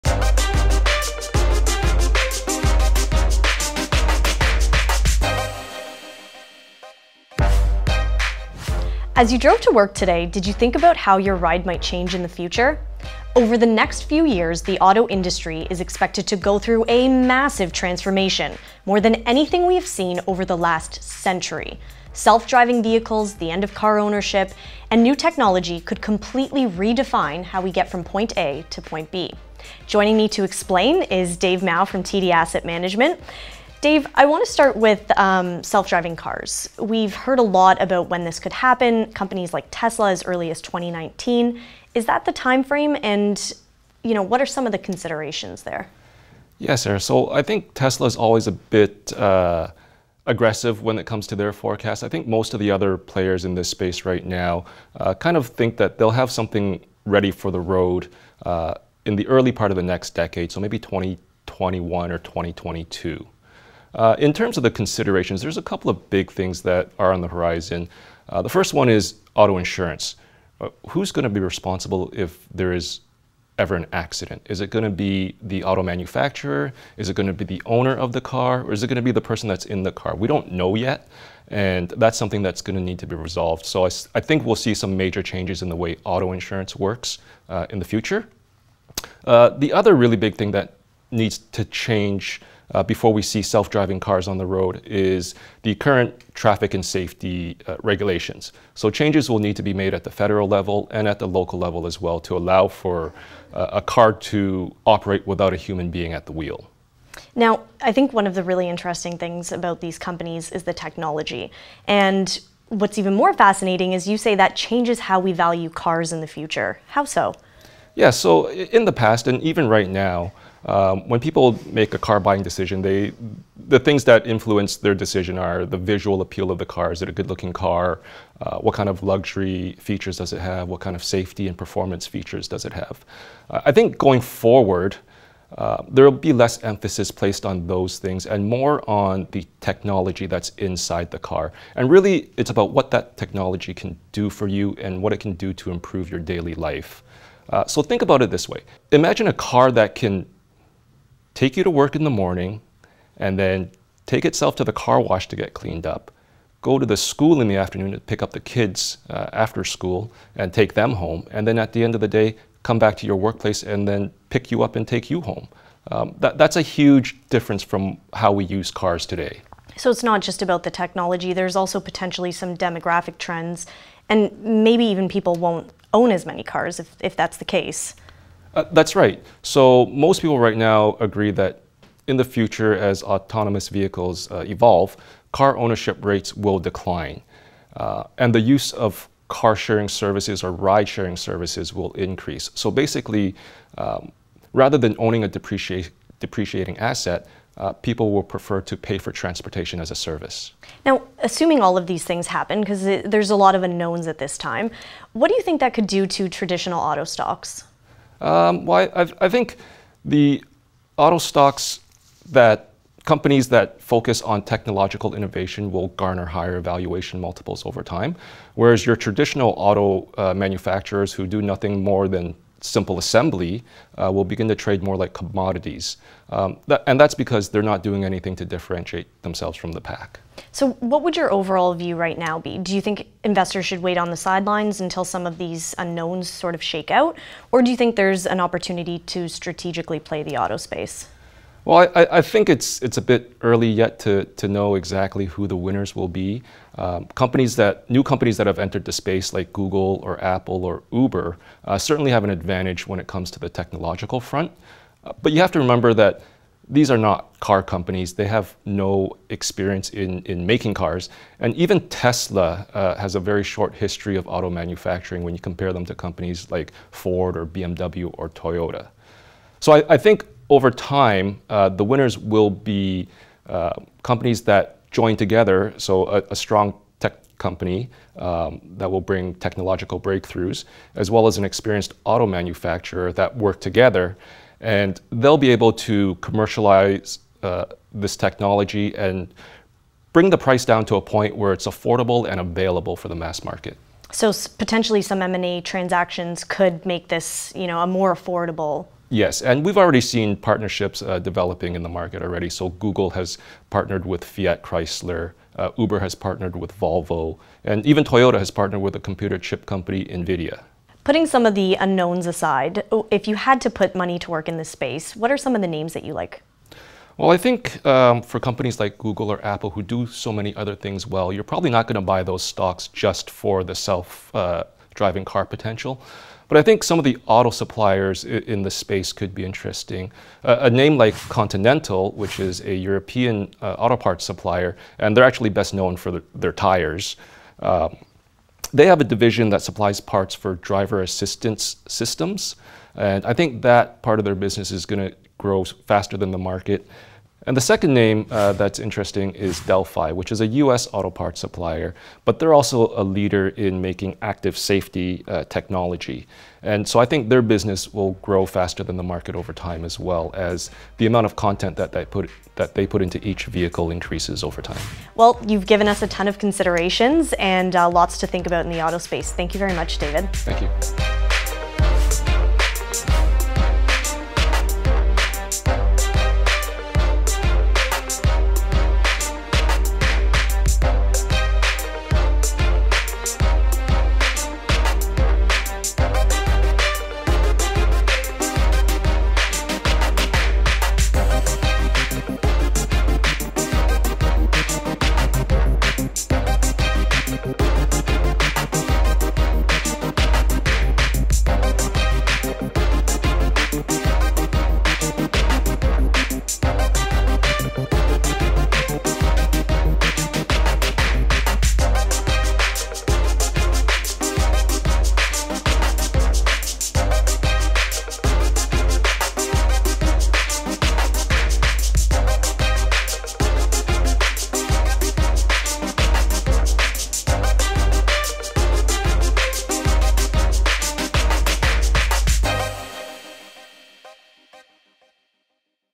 Driving Change: New Beginnings for the Auto Industry
David Mau, Portfolio Manager, TD Asset Management, discusses how self-driving vehicles, the end of car ownership and tech changes could transform how we get from A to B.